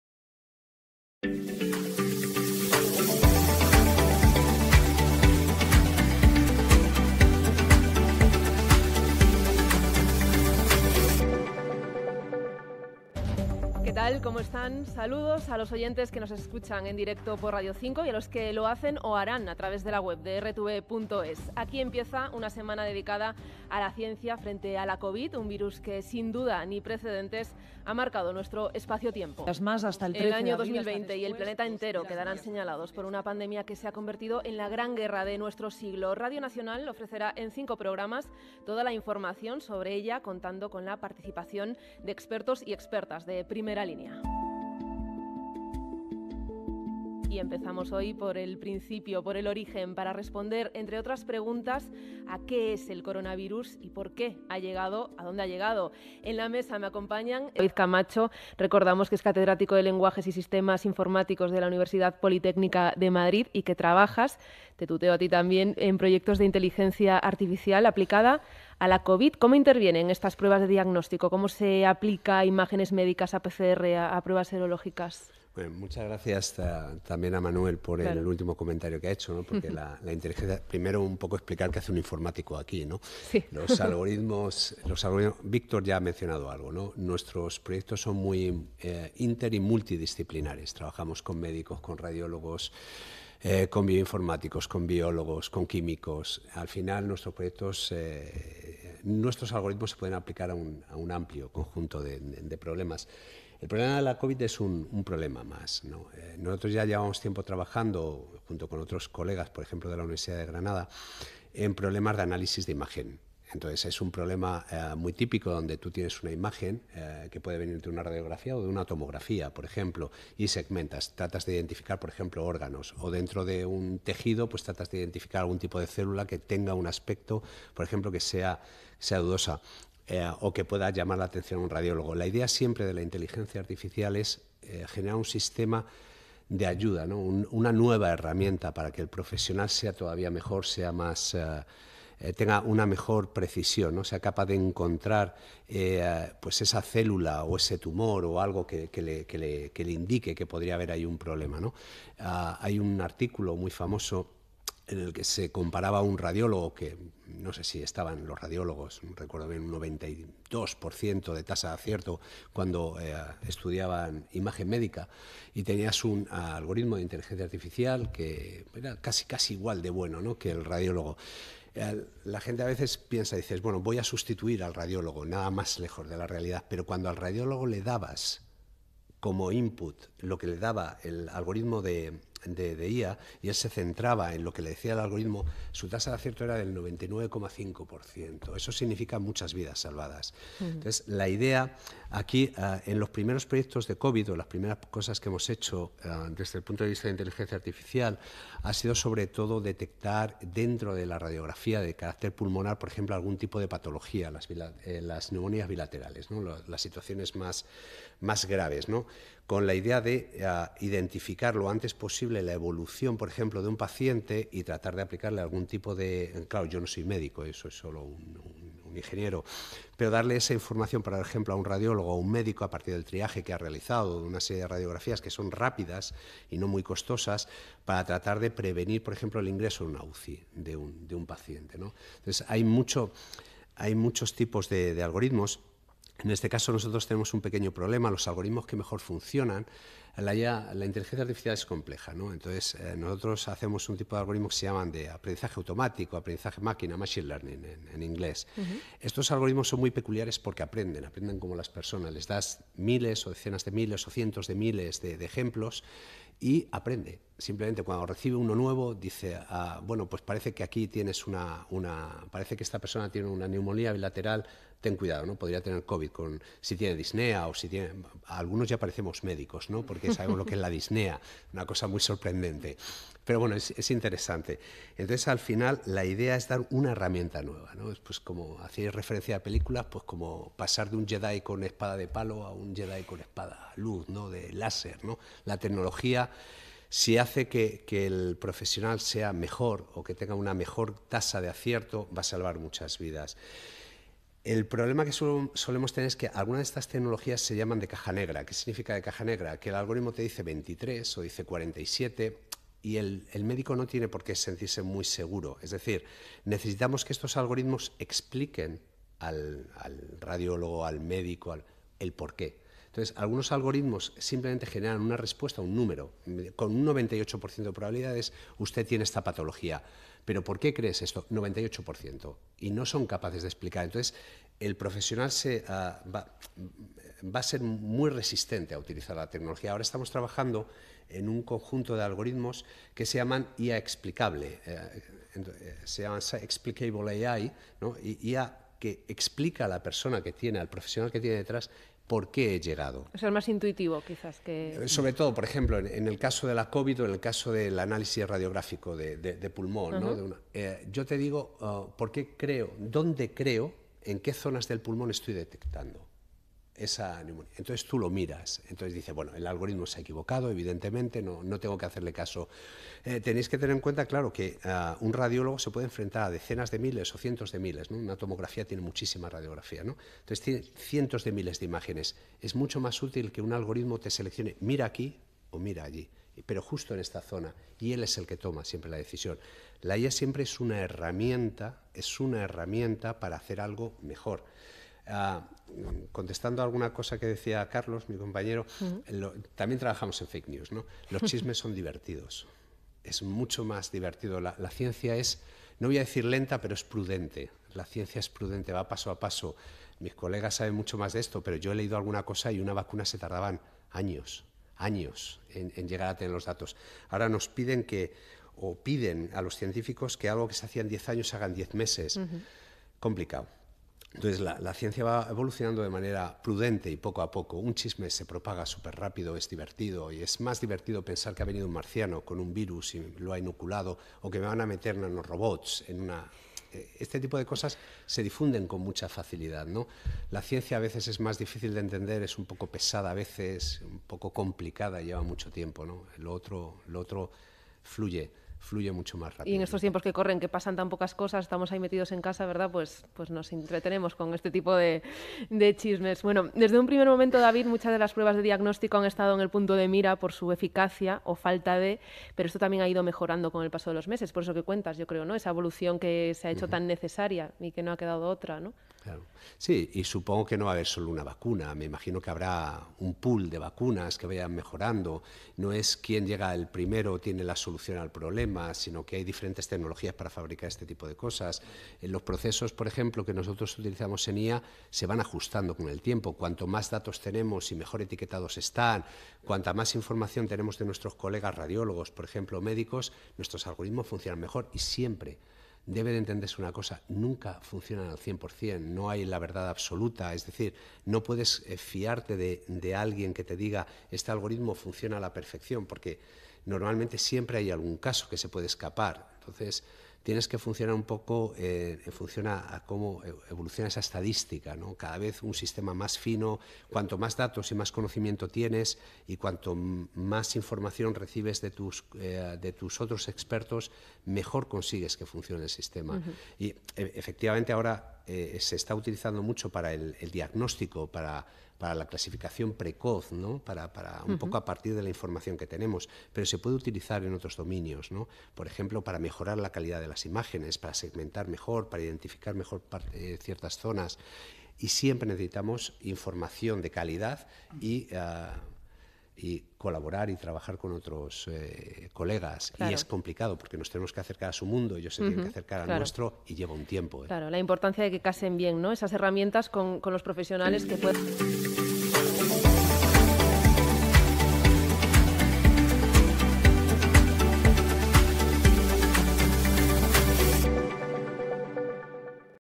David Camacho Fernandez en RTVE
Extracto de la intervención de nuestro compañero Catedrático David Camacho Fernández en Radio Nacional , El covid y la Inteligencia Artificial.